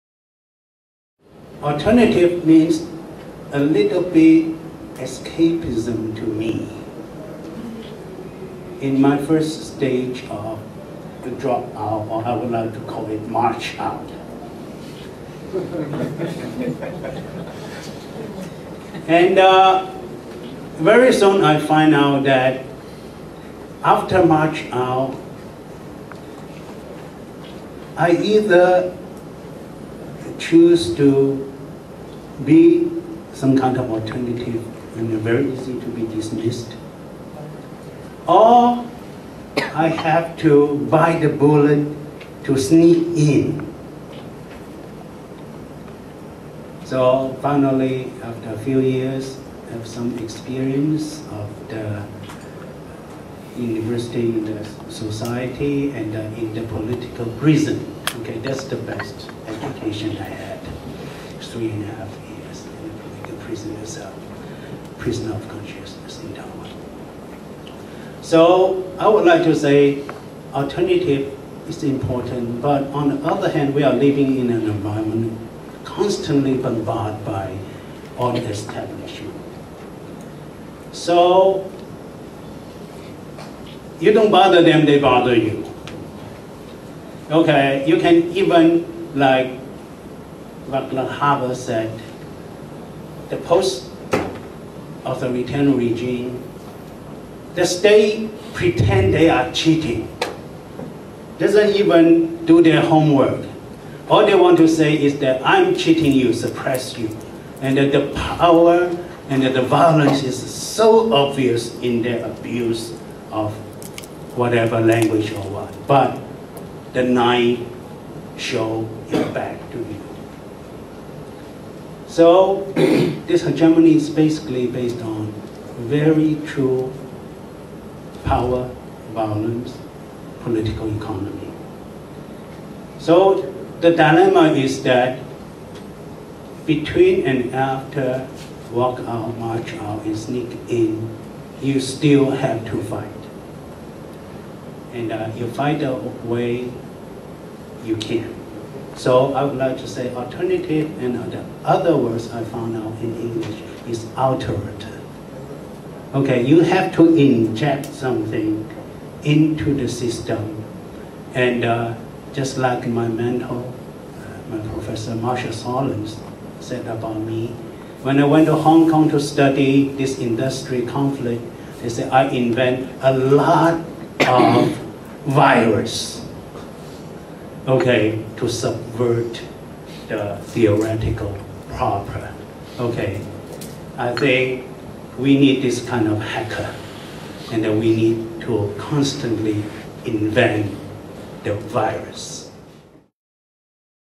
Multiversity - Fred Chiu on Academic Alternatives
How can Third World academics seek intellectual alternatives in the dominant university system?